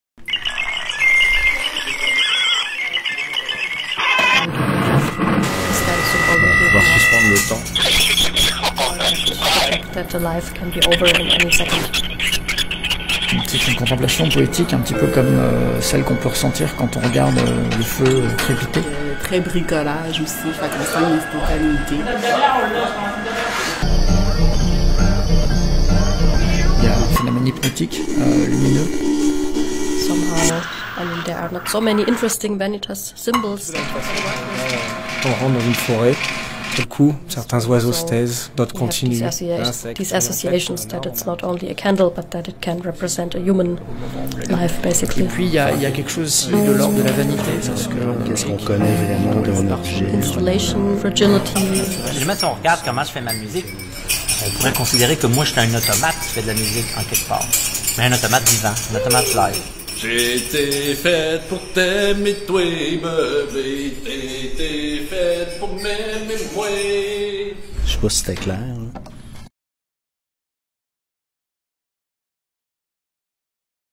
Mois Multi 2019 - Les installations
Conception vidéo : Josiane Roberge

Mois Multi – 20e édition
24 janvier au 3 mars 2019
Le festival international d'arts multidisciplinaires et électroniques de Québec